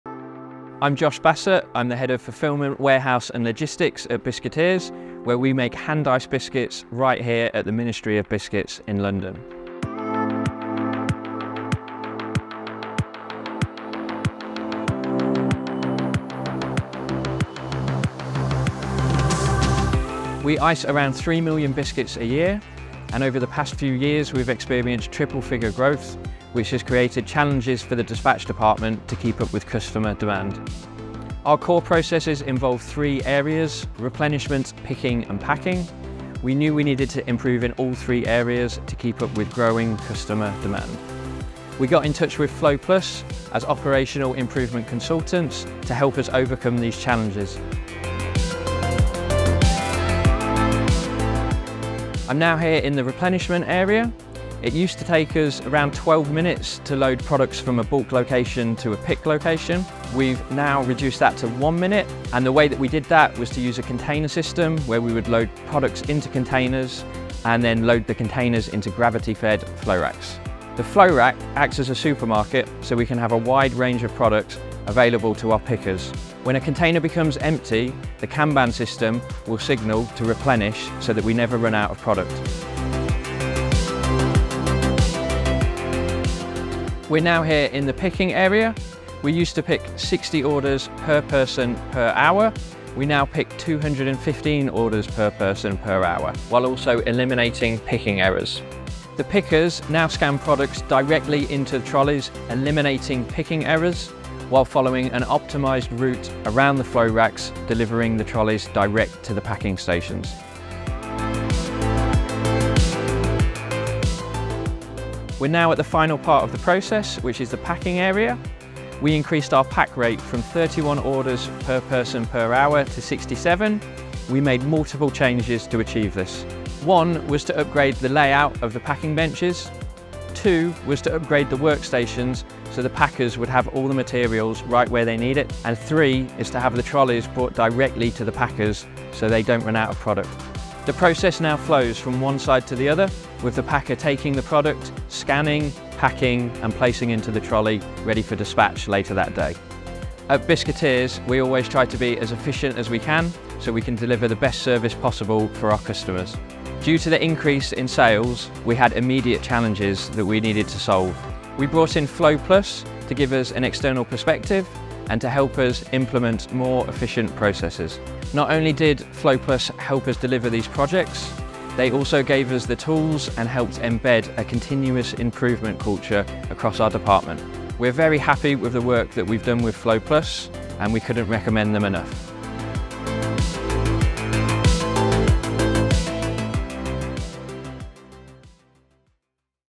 How Biscuiteers Transformed their dispatch department
An inside look into the operational transformation FlowPlus helped Biscuiteers carry out within their dispatch department.

Learn how FlowPlus helped them:
- Increase replenishment efficiency by 12x
- Increase picking efficiency by 350%
- Increase packing efficiency by 115%